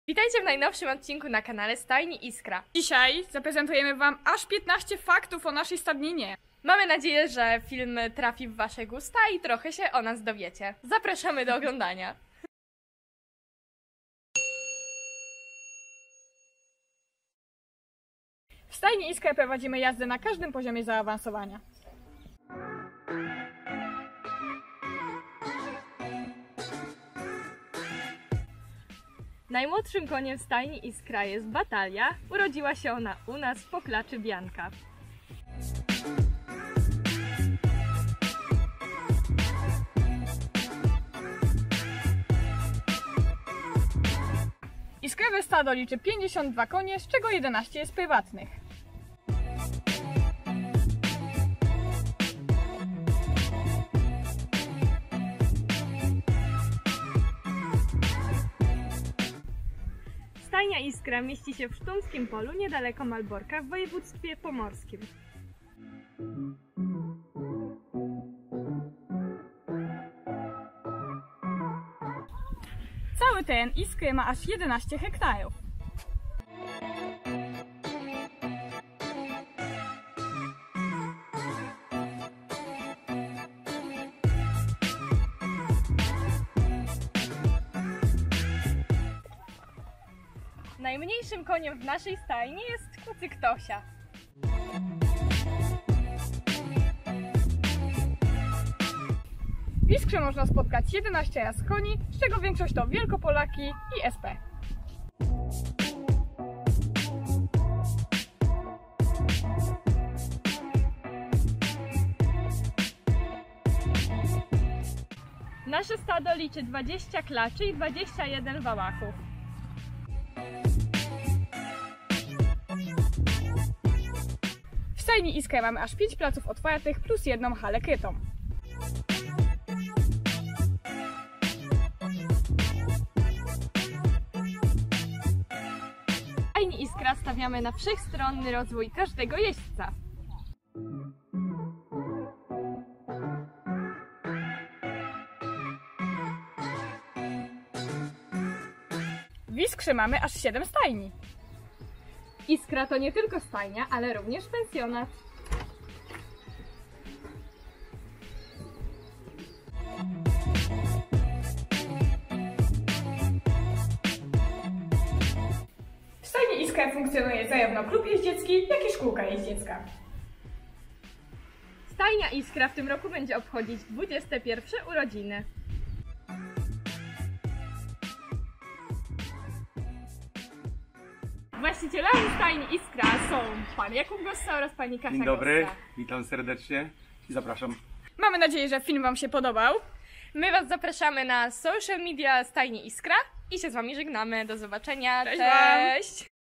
15 FAKTÓW O STAJNI ISKRA | Czy wiesz, że...?
Poznaj 15 najważniejszych  i najciekawszych faktów o Stajni Iskra. Dowiedz się ile mamy koni, jaki koń jest największy oraz kto jest właścicielem Iskry.